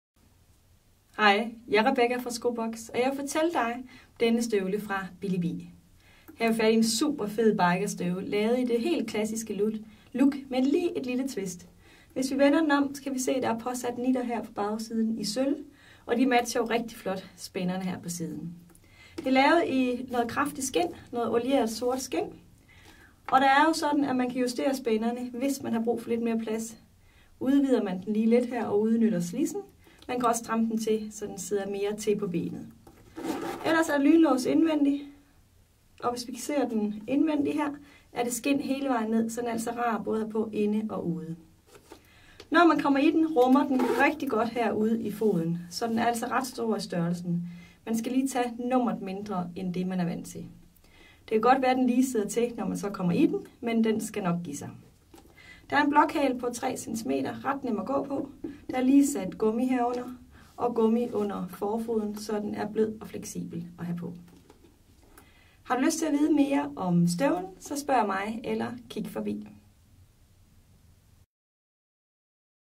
Skobox - Den klassiske Billi Bi bikerstøvle med et lille tvist - Køb Billi Bi bikerstøvler online
- Se endnu flere detaljer med 360 gradersvisningen hvor du også har mulighed for at zoome helt ind.

Hej jeg hedder Rebecca og jeg vil i denne videopræsentation fortælle dig om en Billi Bi Biker støvle. Jeg gennemgår hvilke detaljer og funktioner denne Billi Bi damestøvle har, således det er nemmere for dig, at se om det er de rette Biker støvler du har valgt.